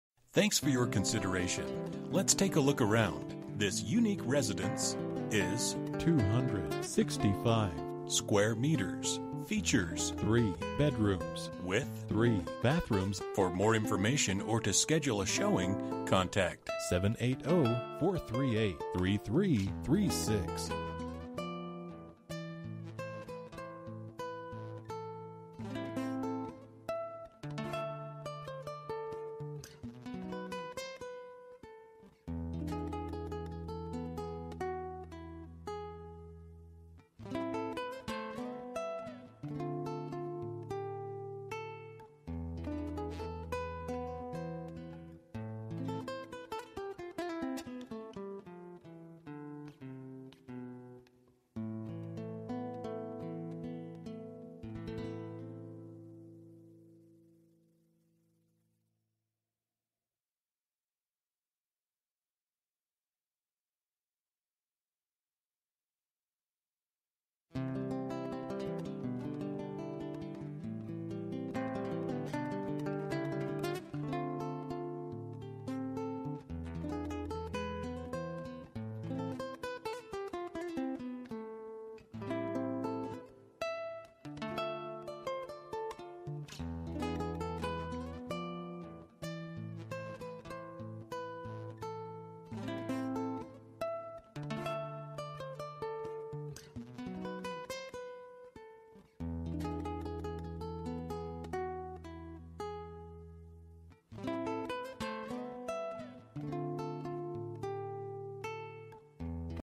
Residential for sale - 632 Wotherspoon Close NW, Edmonton, AB T6M2K2
One of the best locations in Wedgewood! This 2,848 sqft 2 Storey is located in the end of a cul-de-sac which backs onto the expansive 15 acre Wedgewood Park. On the main floor you will find a huge open gourmet kitchen with convection stovetop/oven and granite, a large living space with gas fireplace and office/bedroom with a full bathroom. Upstairs you will have  your private master bedroom overlooking the park and your 10,451 sqft yard, you'll have the option to either have 2 bedrooms and a bonus room or 3 bedrooms as well as an option to move the laundry room upstairs. This beautiful home is equipped with 2 newer hot waters tanks, 2 high efficient furnaces, air conditioning, central vac throughout and a teenagers dream, an unspoilt basement with separate staircase leading to the garage. It is located close to schools K-12, all services, shopping and transportation as well as connecting with the west Edmonton trail system.

Bedrooms: 3
Bathrooms: 3 Bathrooms
Square Feet: 265
Price: $724,900

For more information about this property, please contact Eden & Christopher Hampson at or . You can also text 3297225 to 67299.

04/18/2016 06:49:02 pm

Last modified: